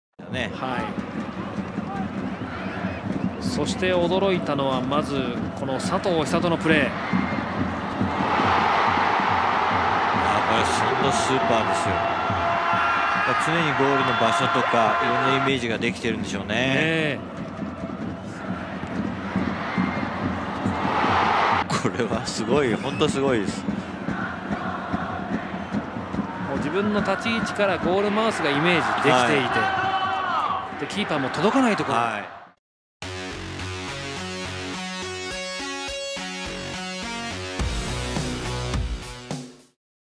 [Football hits] Japanese Soccer Star Hisato Sato Scores Outrageous Spinning Volley Lob
Hisato Sato has scored more than 140 goals during his 14-year career in Japan's J-League. Few, if any can be as good as the one he scored on Saturday. Sato, a 31-year-old star striker for Sanfrecce Hiroshima, scored one of world soccer's best goals this weekend in his team's 2-1 victory over against Kawasaki Frontale. With his team down 1-0 in the 57th minute, Sato flicked the ball up to himself and shot straight into the corner of the goal — despite the fact that he was facing his own goal when he started the move. It was Sato's first goal of the nascent J-League season. Good luck trying to score a better one in the coming months.